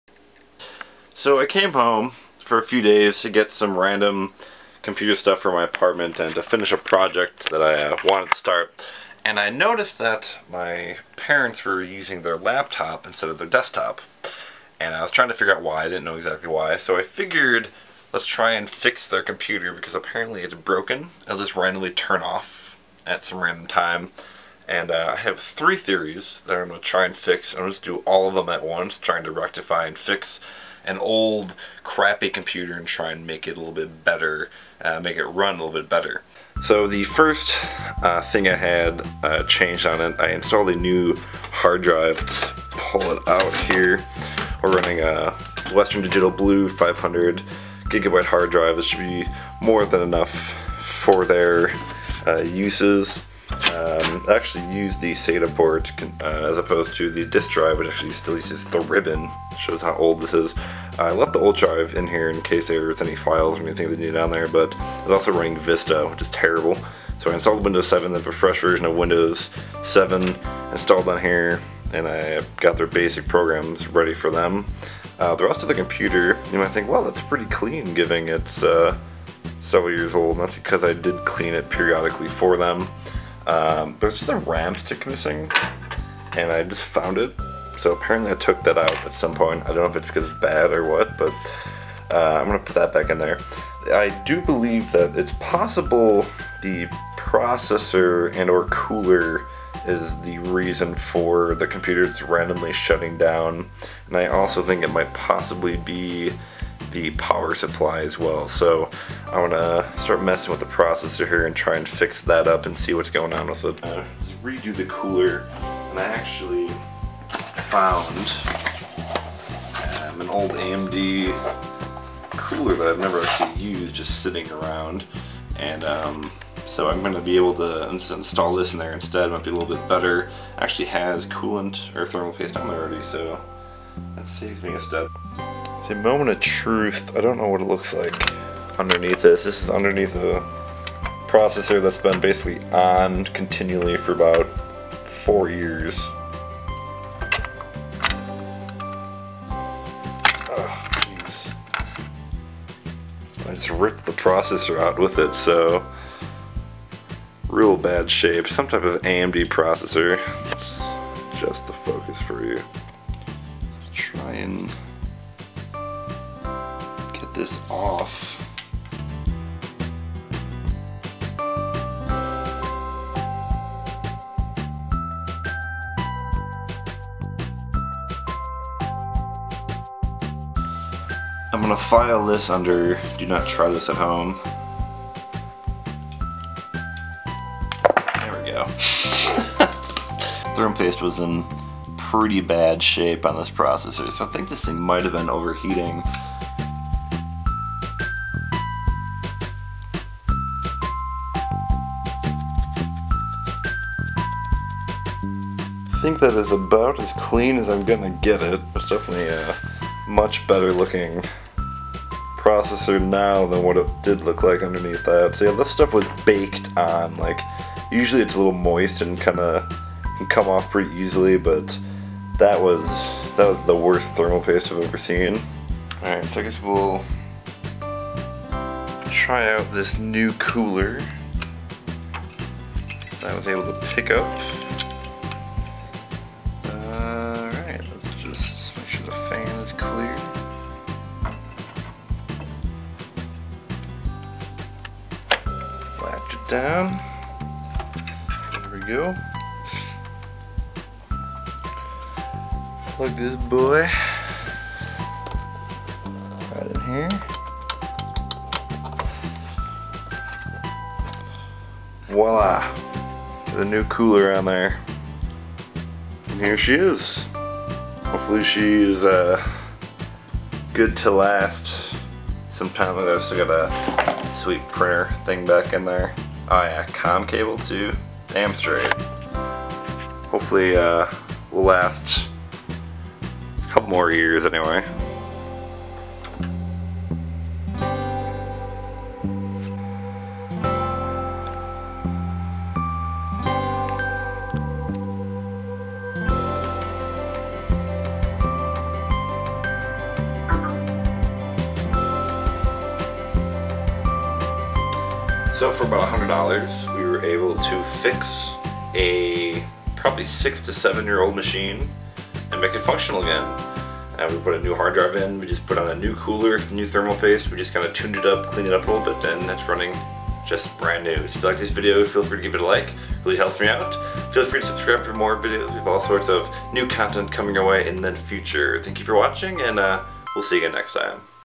Giving a 7-year-old computer a second life
Fixing up and giving life to a 7-year-old computer. Basically a quick little vlog as I do some quick upgrades to make an old computer run like "new" again. This can be useful to get basic computers running again. Obviously, this isn't a mega upgrade, but for online usage and word processing, it works great.